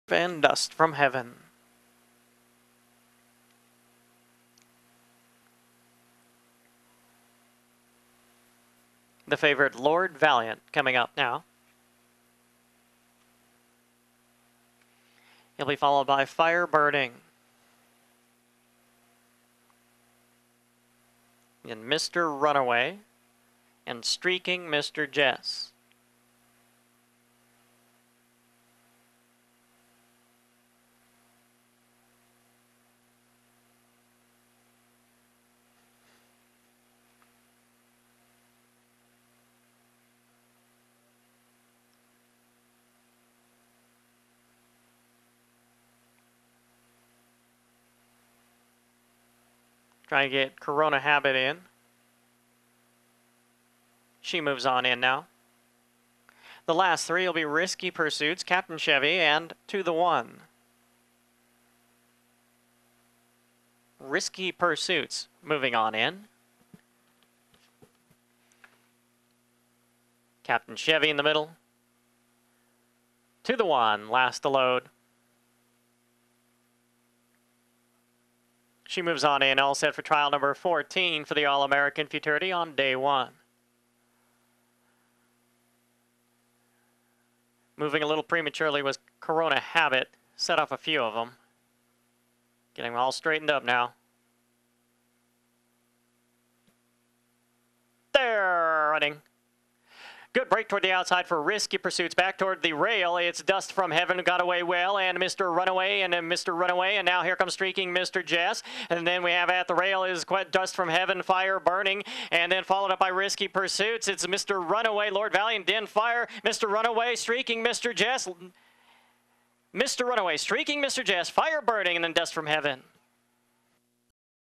AA Futurity Trial 14 August 15, 2013
1. Corona Habit :21.55
2. Streaking Mr Jess
3. Fire Burning
4. Dust From Heaven